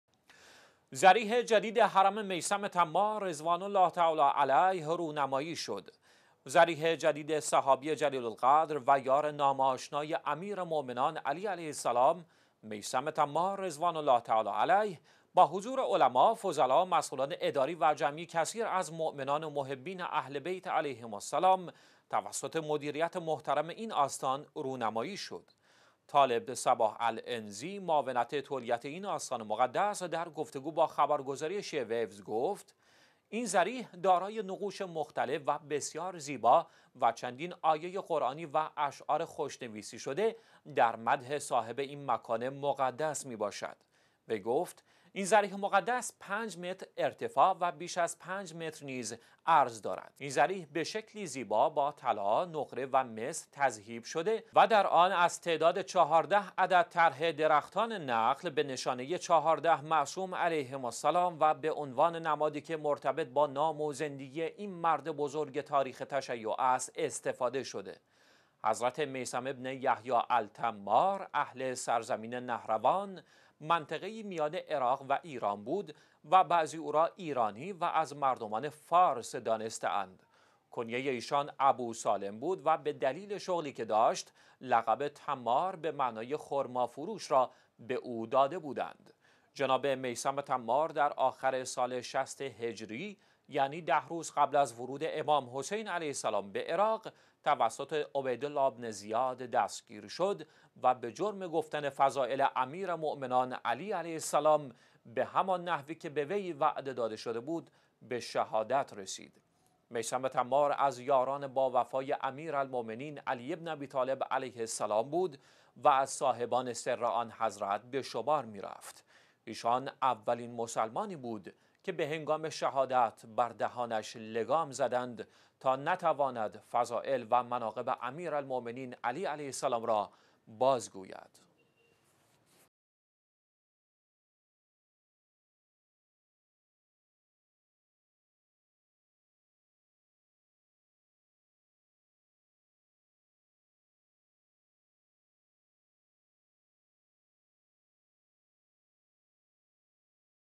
مراسم افتتاح ضریح مطهر صحابی جلیل القدر «میثم تمار» رضوان الله علیه/ ۲۹ اسفند ۱۳۹۵
مراسم افتتاح ضریح مطهر صحابی جلیل القدر «میثم تمار» رضوان الله علیه

ضریح جدید حرم «میثم_تمار» (رضوان الله تعالی علیه)، رونمایی شد.

ضریح جدید صحابی جلیل القدر و یار نام آشنای «امیرالمؤمنین علیه السلام»، «میثم تمار»رضوان الله تعالی علیه، با حضور علما ،فضلا، مسئولان اداری و جمعی کثیر از مومنان و محبین اهل بیت علیهم السلام، توسط مدیریت محترم این آستان، رونمایی شد.

«طالب صباح العنزی» معاونت تولیت این آستان مقدس، در گفتگو با خبرگزاری شیعه ویوز، گفت: این ضریح دارای نقوش مختلف و بسیار زیبا، و چندین آیه قرانی و اشعار خوش نویسی شده در مدح صاحب این مکان مقدس می باشد.

وی گفت: این ضریح مقدس 5 متر ارتفاع و بیش از 5 متر نیز عرض دارد. این ضریح به شکلی زیبا با طلا ، نقره و مس تذهیب شده و در آن از تعداد 14 عدد طرح درختان نخل، به نشانه چهارده معصوم علیهم السلام و به عنوان نمادی که مرتبط با نام و زندگی این مرد بزرگ تاریخ تشیع است، استفاده شده.

حضرت «میثم بن یحیی التمار» اهل سرزمین «نهروان» ـ منطقه‌ای میان عراق و ایران ـ بود و بعضی او را ایرانی و از مردمان فارس دانسته‌اند.

کنیه ایشان «ابو سالم» بود و به دلیل شغلی که داشت، لقب «تمار» به معنی خرما فروش را به او داده بودند.

جناب «میثم تمار» در آخر سال ۶۰ هجری، یعنی ده روز قبل از ورود امام_حسین علیه السلام به عراق؛ توسط «عبیدالله بن زیاد» دستگیر شد و به جرم گفتن فضائل امیرالمومنین علی علیه السلام، به همان نحوی که به وی وعده داده شده بود، به شهادت رسید.

«میثم تمار» از یاران با وفای «امیرالمؤمنین علی بن ابی طالب علیه السلام» بود و از صاحبان سرِّ آن حضرت به شمار می رفت.

ایشان اولین مسلمانی بود که به هنگام شهادت بر دهانش لگام زدند تا نتواند فضائل و مناقب امیرالمومنین علی علیه السلام را بازگوید.

یکشنبه - ۲۹ اسفند ۱۳۹۵
الأحد - ٢٠ جمادي الثانيه ١٤٣٨